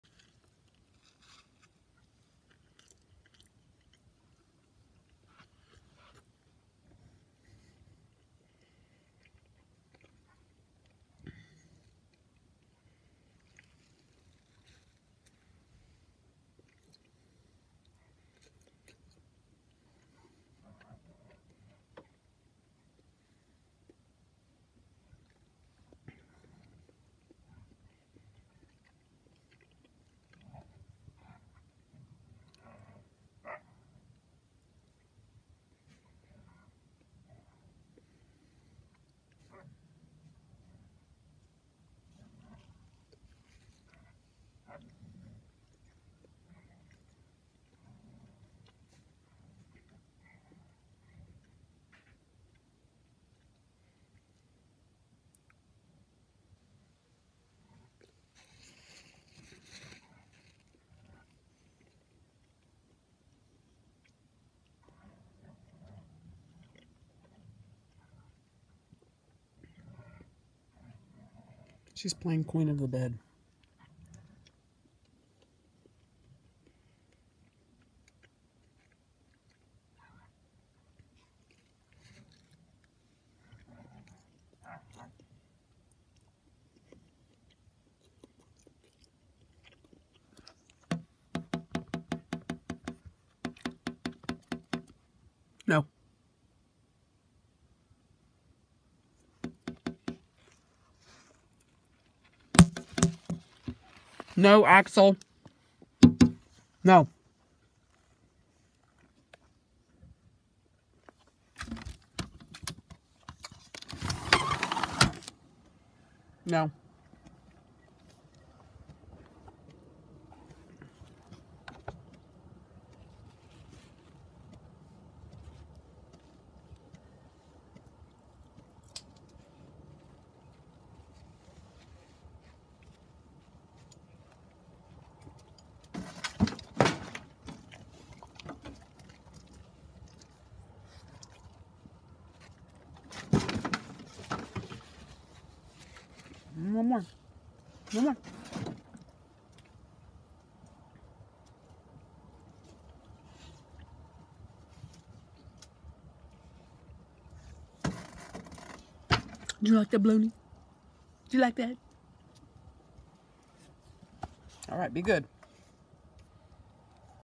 Great Pyrenees puppies x3 playing Queen of their bed. Jinger is the Queen
Addie, Jinger & Axle playing for boss spot on bed